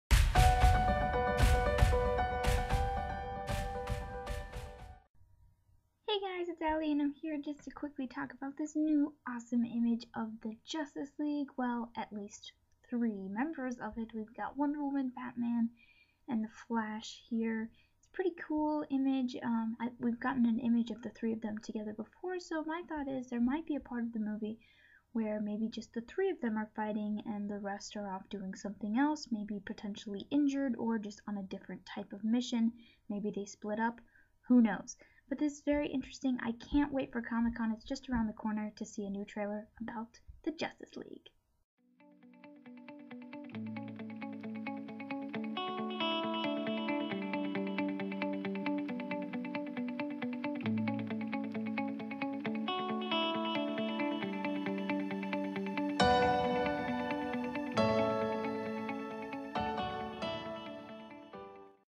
New Justice League Photo Has Me Wondering
Hey guys! Here is some quick analysis of the JL image and it has me wondering about a possible plot in the upcoming film. Enjoy! :) - Aly